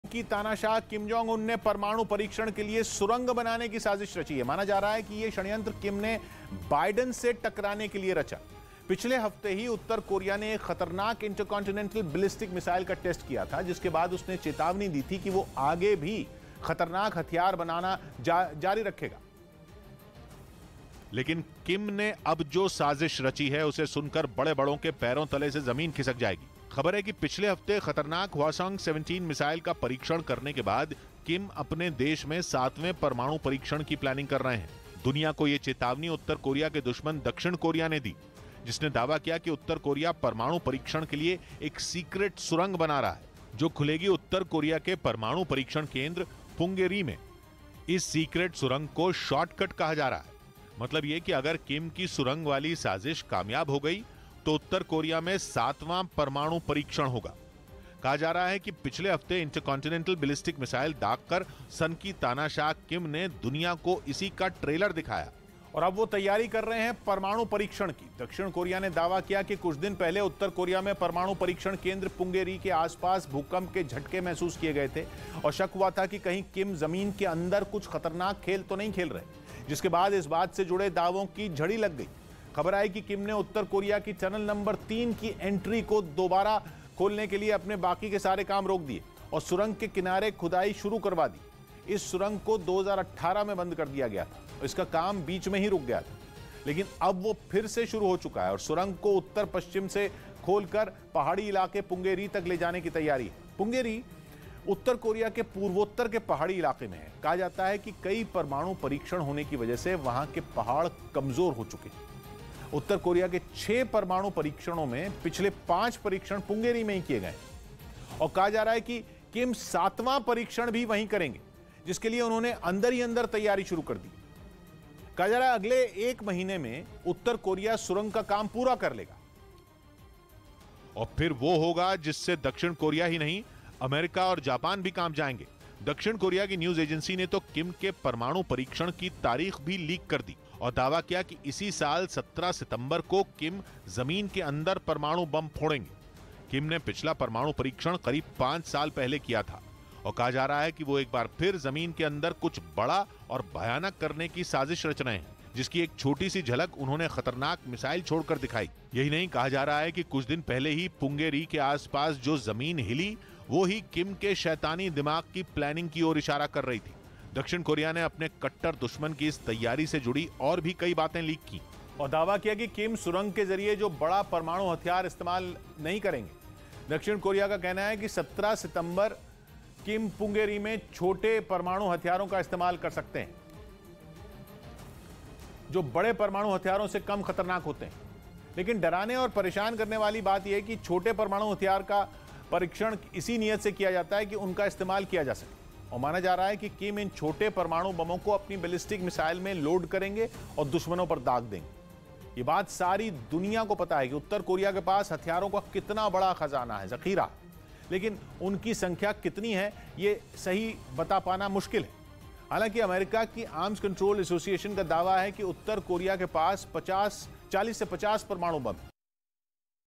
North Korea : Kim Jong Un का परमाणु 'प्लैन', किम की सुरंग वाली साज़िश
North Korea के तानाशाह  Kim Jong Un ने एक ऐसा षडयंत्र रचा है, कि Biden की रातों की नींद उड़ जाएगी। पिछले हफ़्ते ख़तरनाक मिसाइल दागने वाले किम के तेवरों से बाइडन वैसे ही परेशान हैं और अब ख़बर है कि किम ने उत्तर कोरिया का सातवां परमाणु परीक्षण करने का प्लैन बना लिया है।